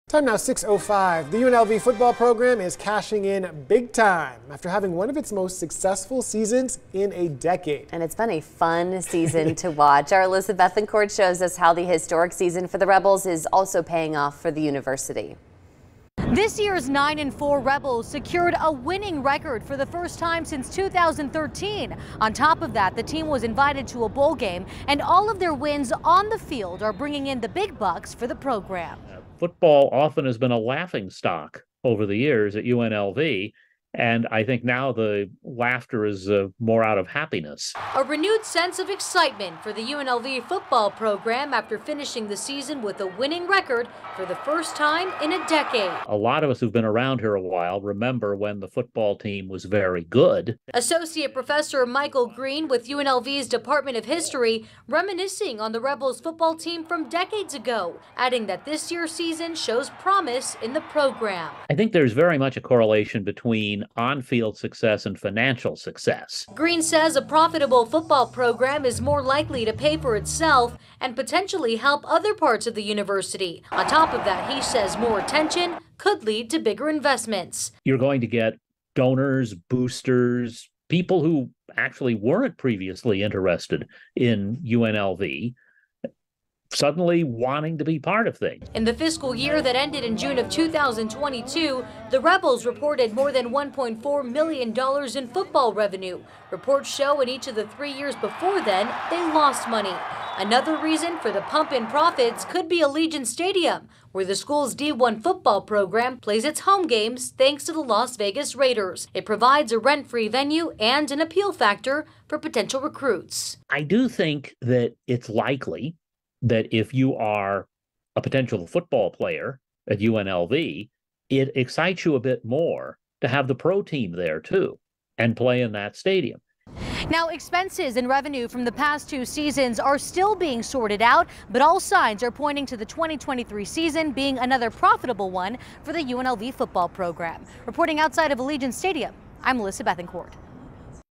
UNLV football team’s success could mean big profits for the program
Our Alyssa Bethencourt tells us how the UNLV football program could cash in big time after playing one of its most successful seasons in a decade.